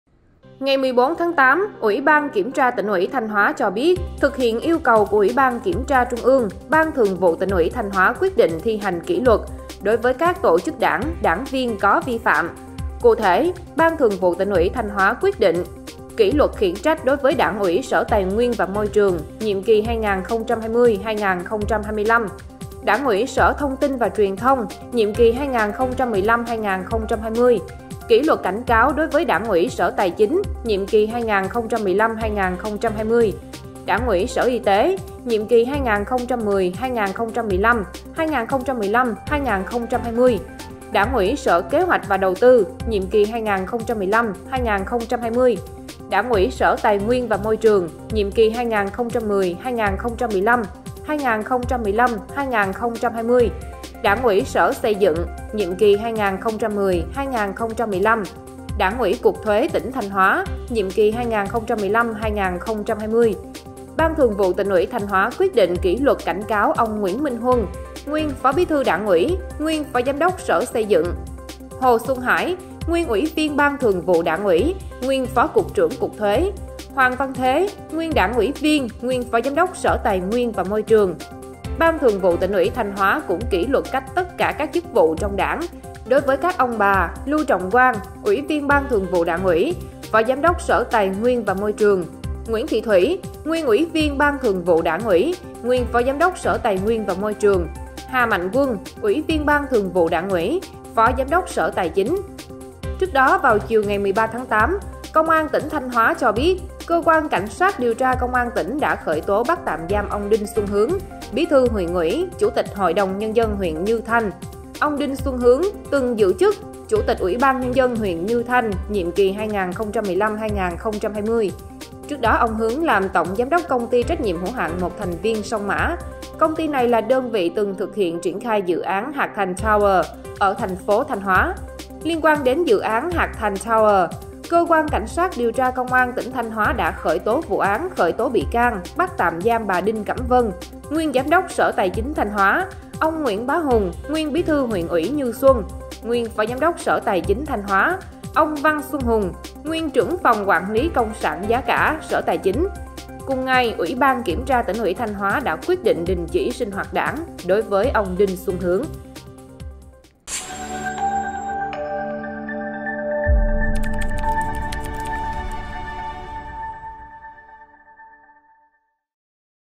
Thanh Hóa: Kỷ luật một số tổ chức Đảng và lãnh đạo; Bắt tạm giam Bí thư Huyện ủy Như Thanh
Ngày 14-8, Ủy ban Kiểm tra Tỉnh ủy Thanh Hóa cho biết, thực hiện yêu cầu của Ủy ban Kiểm tra Trung ương, Ban Thường vụ Tỉnh ủy Thanh Hóa quyết định thi hành kỷ luật, đối với các tổ chức Đảng, đảng viên có vi phạm.

Trước đó, vào chiều 13-8, Công an tỉnh Thanh Hóa cho biết, Cơ quan Cảnh sát điều tra Công an tỉnh đã khởi tố, bắt tạm giam ông Đinh Xuân Hướng, Bí thư Huyện ủy, Chủ tịch HĐND huyện Như Thanh. Ông Đinh Xuân Hướng từng giữ chức Chủ tịch UBND huyện Như Thanh nhiệm kỳ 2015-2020.